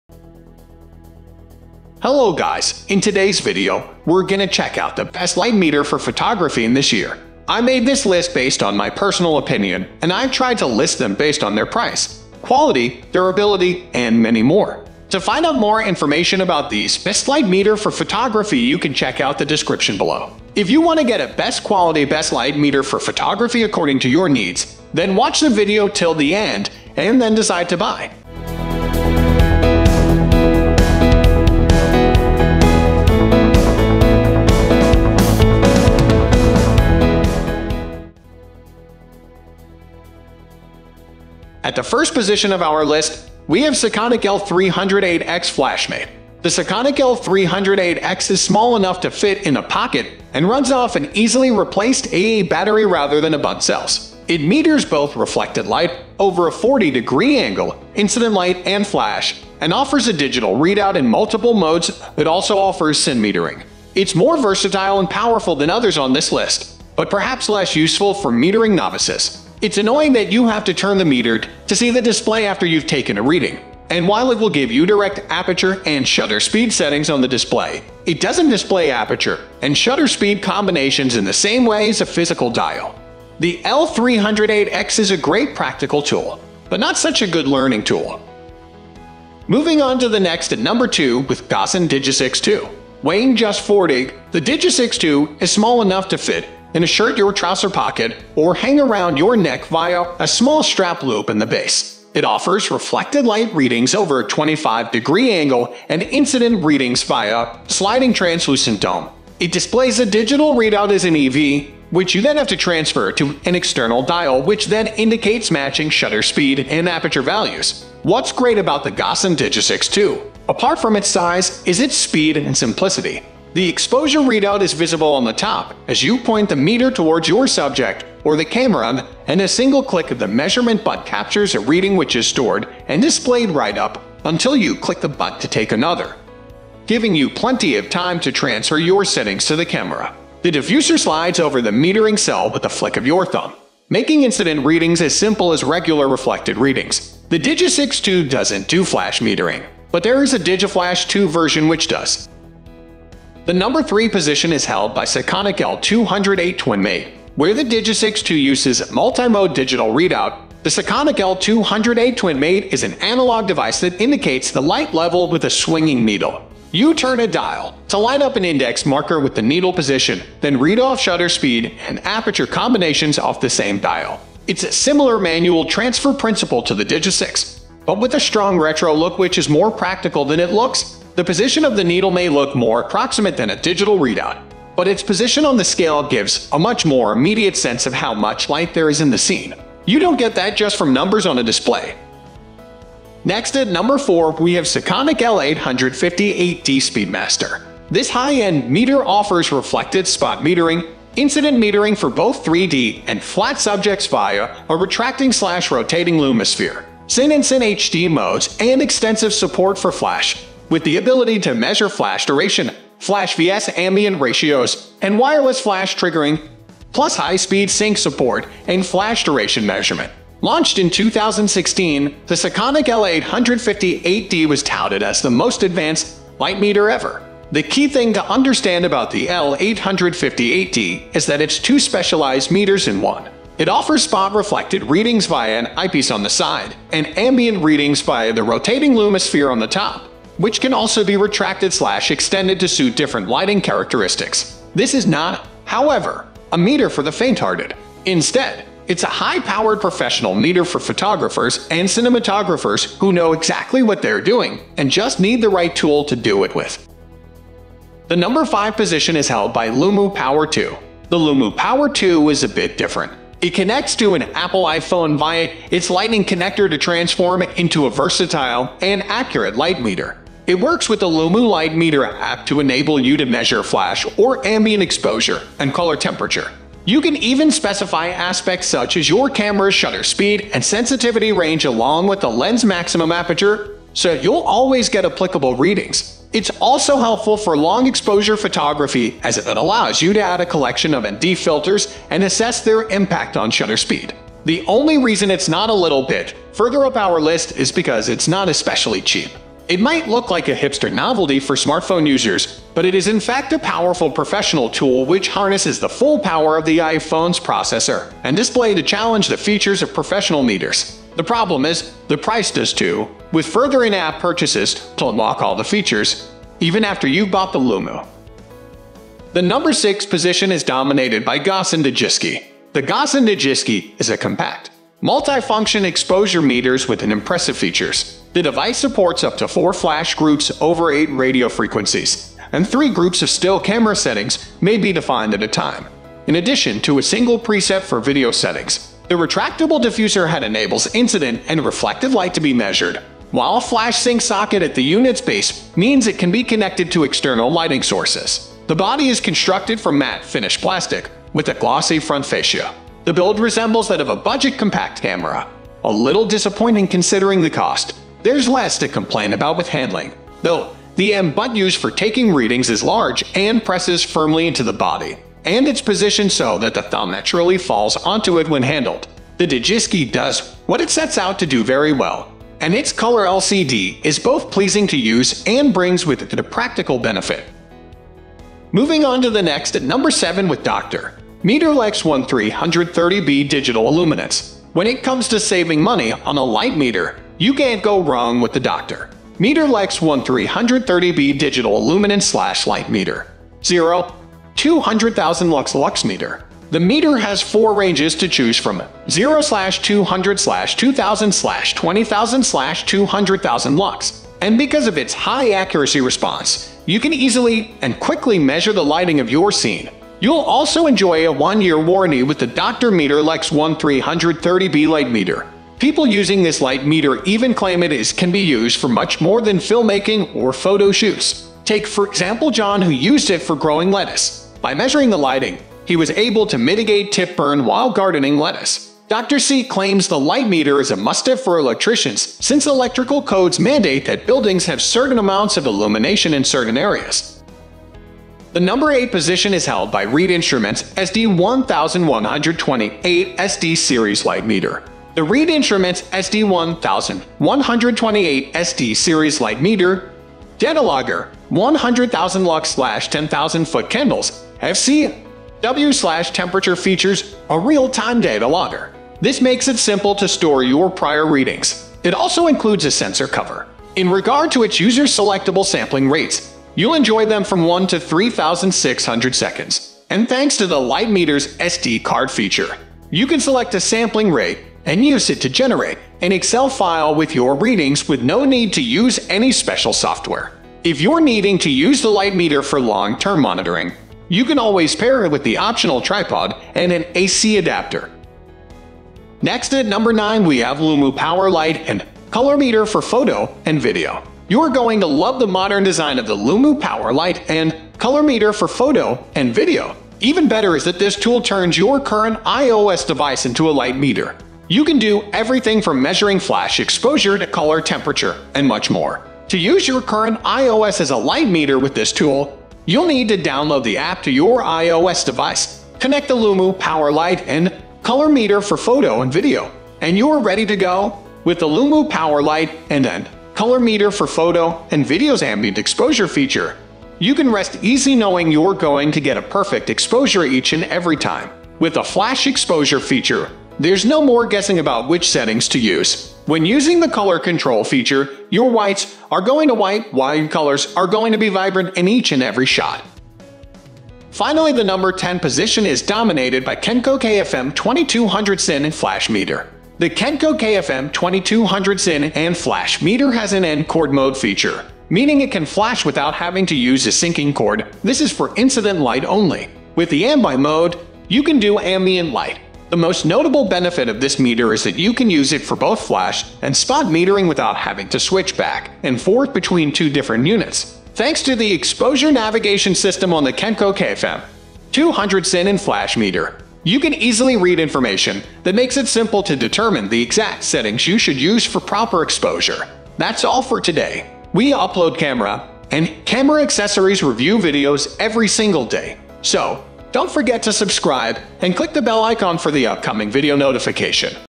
Light Meter for Photography: ✅ Best Photography Light Meter 2023 (Buying Guide)
Links to the Best Photography Light Meter we listed for Any Budget:

1 . Sekonic L-308X Flashmate

2 . Gossen Digisix 2

3 . Sekonic L-208 Twin Mate

4 . Sekonic L-858D Speedmaster

5 . Lumu Power 2

6 . Gossen Digisky

7 . Dr.Meter LX1330B Digital Illuminance

8 . REED Instruments SD-1128 SD Series Light Meter

9 . Lumu Power Light & Color Meter for Photo and Video

10 . Kenko KFM-2200 Cine and Flash Meter

What are the Best Photography Light Meter in 2023?

In today's video we reviewed the Top 10 best Photography Light Meter on the market in 2023. We made this list based on our personal opinion and we ranked them in no particular order, after doing our research based on their prices, quality, durability, brand reputation and many more.

After watching this video you will know which are the best budget, top selling and top rated Photography Light Meter on the market today.

If you choose from this list you can be sure that you buy one of the best Photography Light Meter available today.